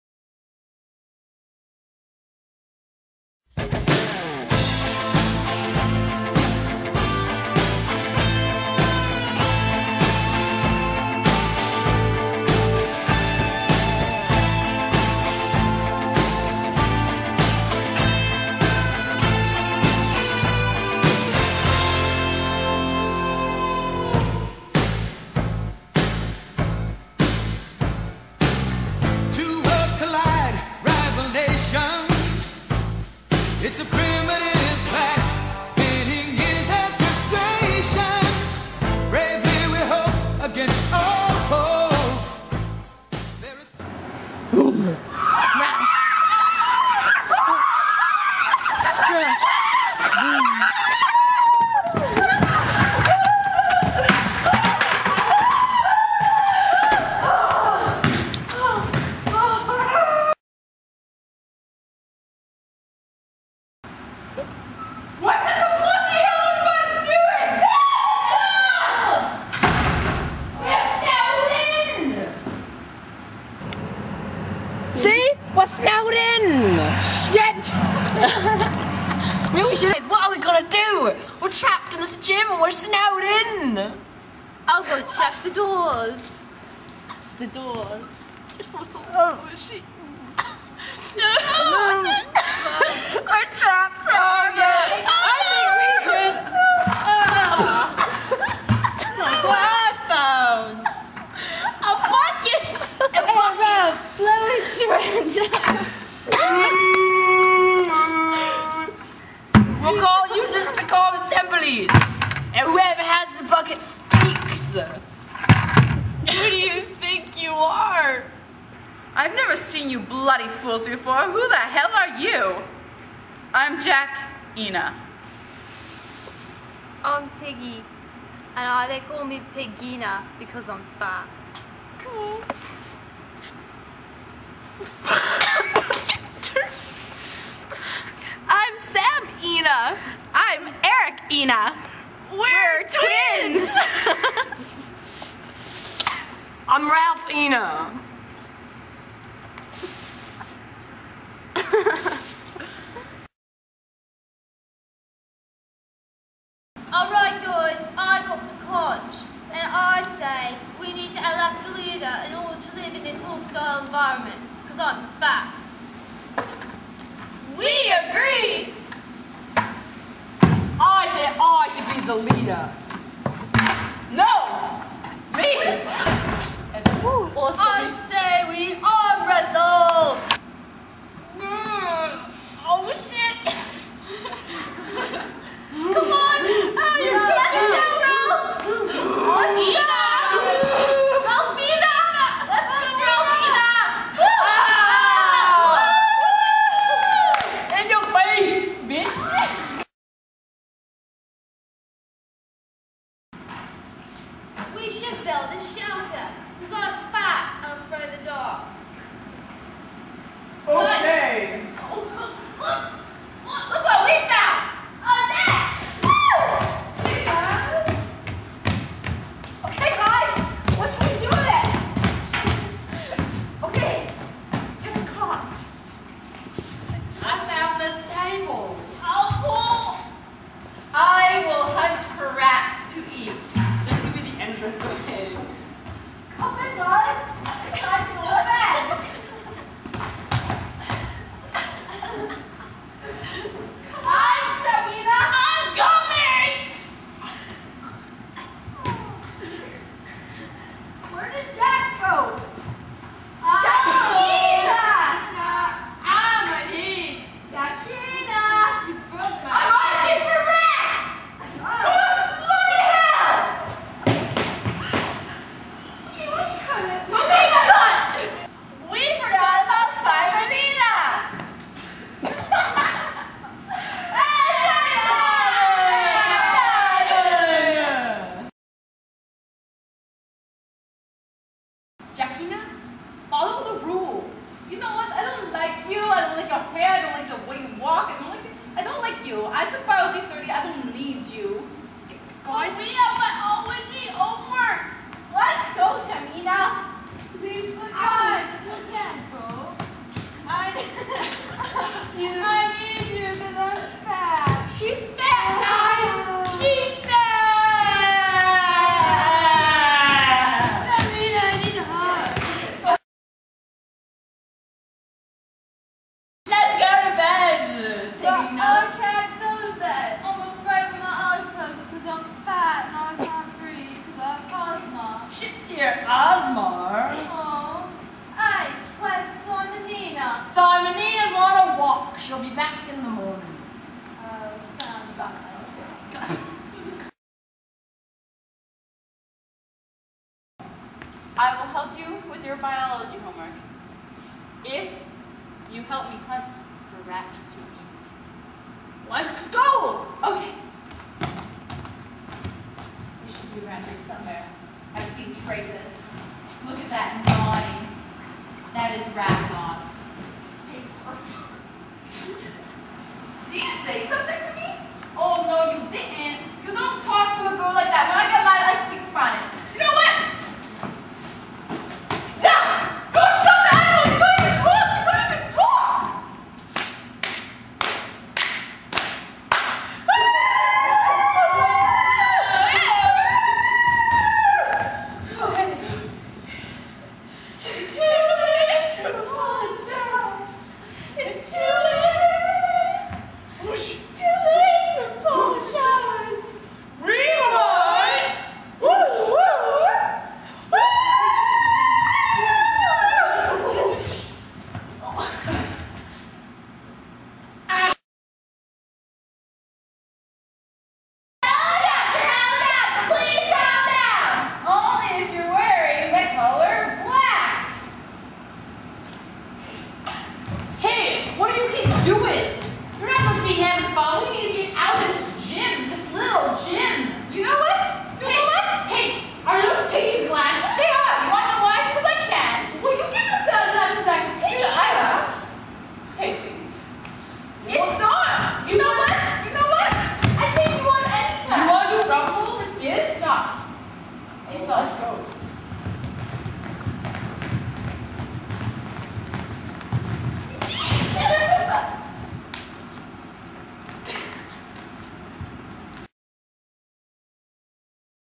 Lord of the Litter Gym- Part 1
This is a class project for my english class. This is our version of Lord of the Flies. We are crafty people when it snows! So clearly it is going to be better.